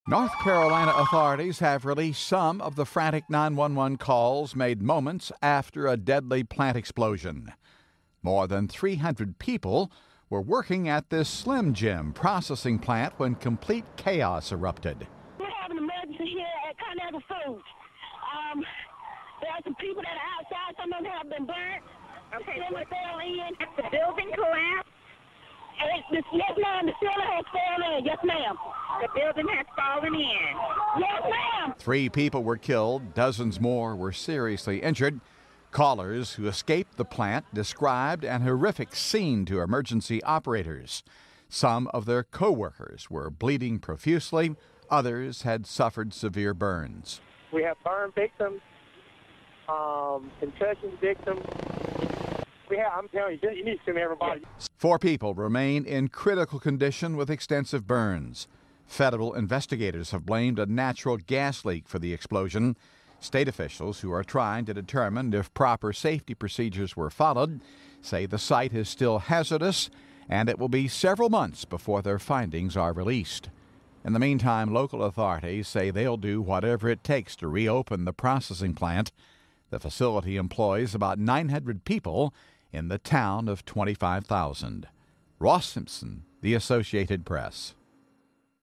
911 Tapes Detail Panic After NC Plant Blast
Emergency tapes released Monday show workers dialed 911 in a frenzy after escaping an explosion that killed three workers at a Slim Jim processing plant, with one panicked woman telling dispatchers: 'ConAgra just blew up.' (June 15)